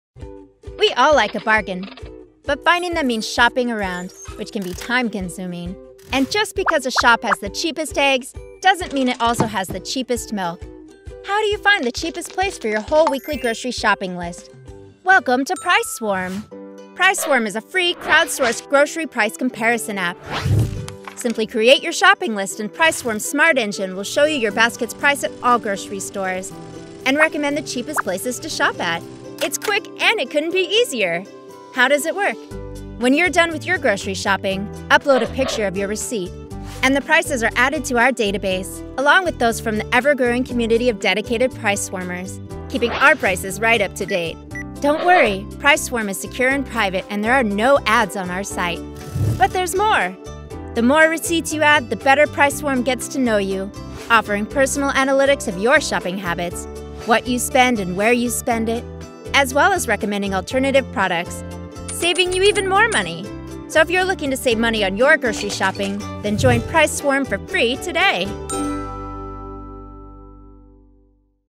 Grocery Price Comparison App | 2D Animation | Video Animation
Get the best Grocery prices by comparing at the PriceSwarm Grocery Comparison App.